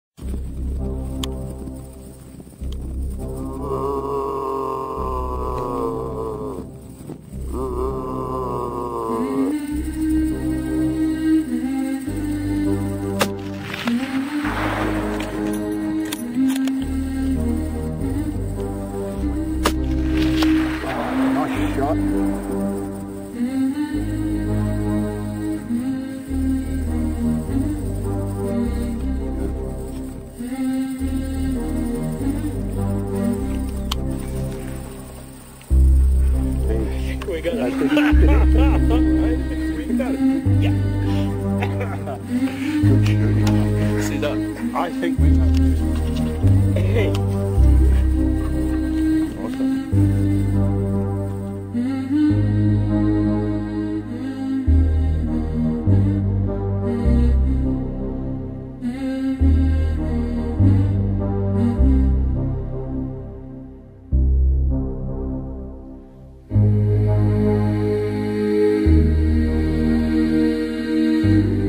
Hunting Red Stag with Norbert.wmv
Hunting Red Stag at Leithen Valley NZ